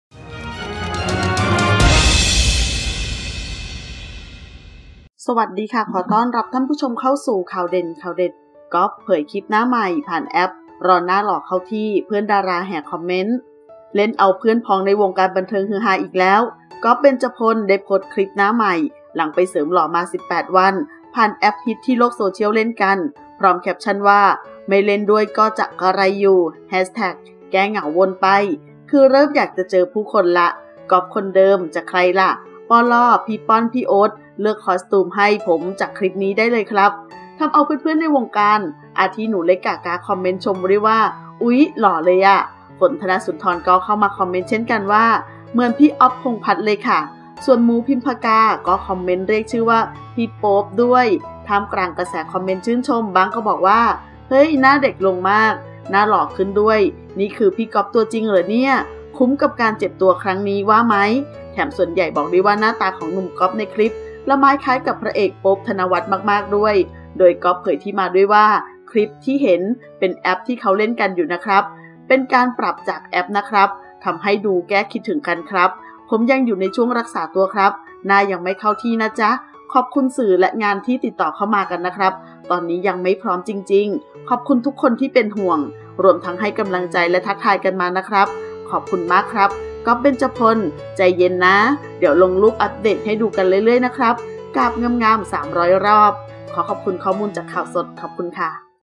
กอล์ฟ เผย หน้าใหม่ ผ่านแอพฯ รอหน้าหล่อเข้าที่ เพื่อนดาราแห่คอมเมนต์
ขอบคุณที่มา :
ขอขอบคุณข้อมูลจาก: